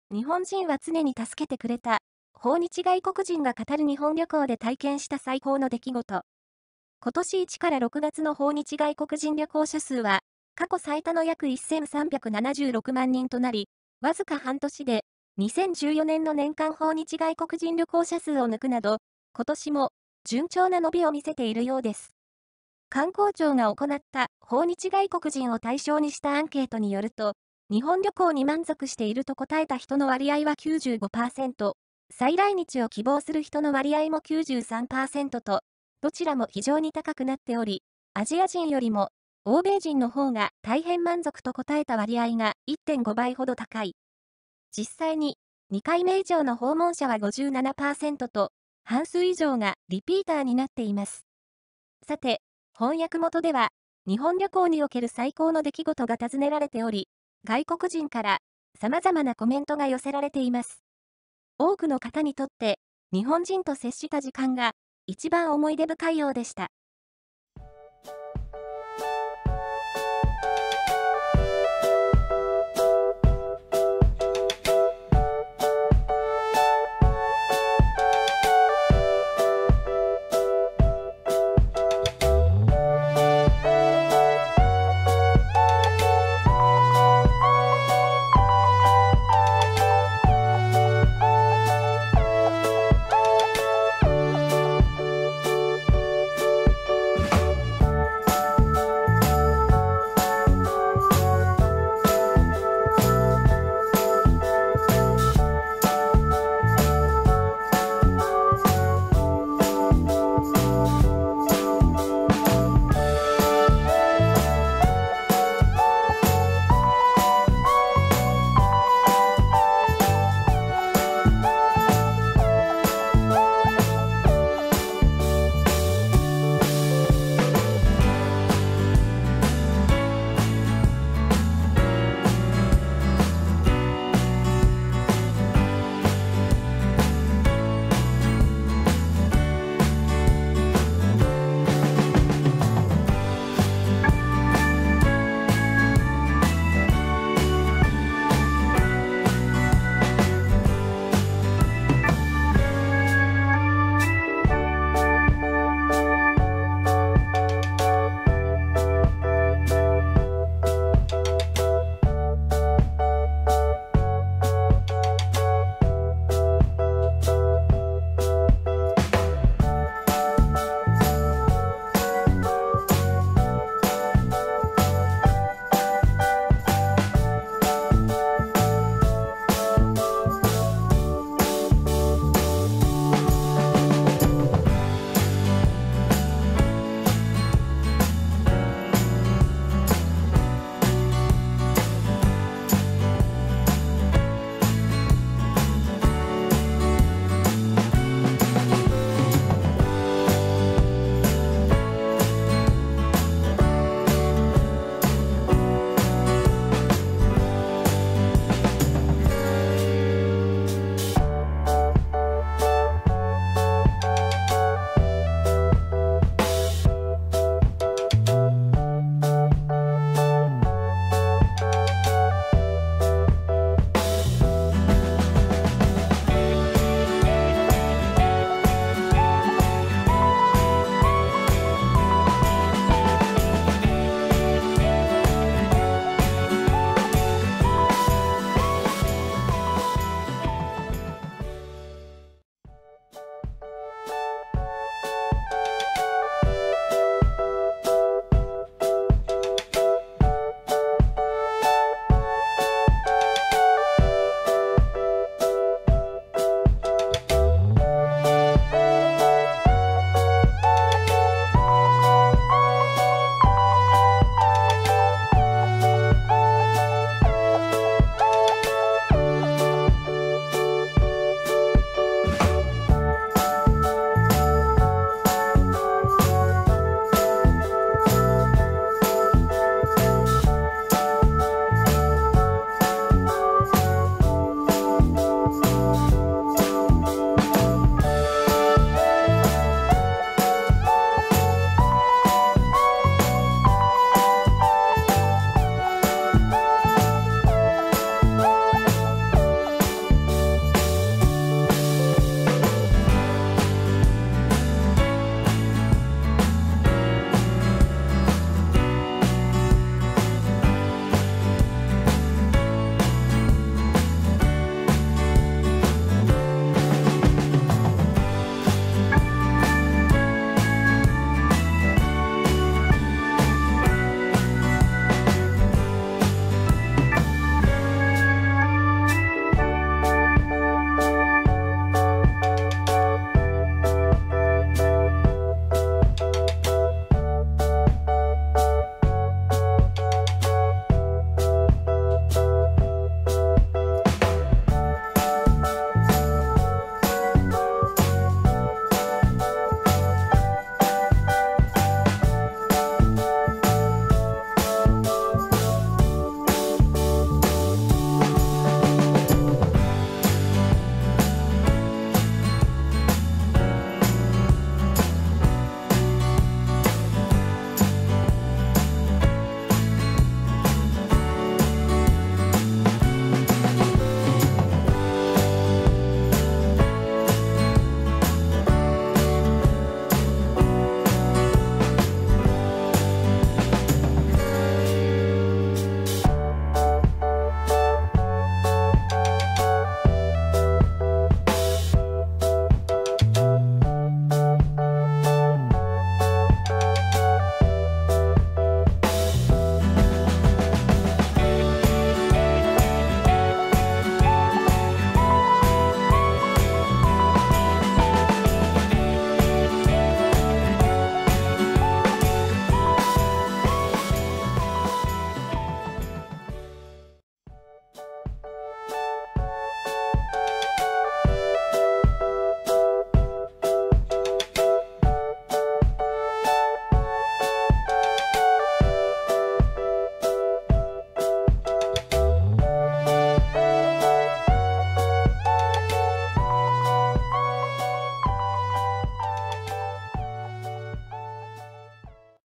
【海外の反応】日本人は常に助けてくれた」 訪日外国人が語る日本旅行で体験した最高の出来事
今年1～6月の訪日外国人旅行者数は過去最多の約1376万人となり、わずか半年で2014年の年間訪日外国人旅行者数を抜くなど、
今年も順調な伸びを見せているようです。
観光庁が行った訪日外国人を対象にしたアンケートによると、
「日本旅行に満足している」と答えた人の割合は95％、
「再来日を希望する」人の割合も93％と、どちらも非常に高くなっており、（アジア人よりも欧米人の方が「大変満足」と答えた割合が1.5倍ほど高い）。実際に2回目以上の訪問者は57％と、半数以上がリピーターになっています。
さて、翻訳元では「日本旅行における最高の出来事」が訊ねられており、外国人から様々なコメントが寄せられています。
多くの方にとって、日本人と接した時間が、一番思い出深いようでした。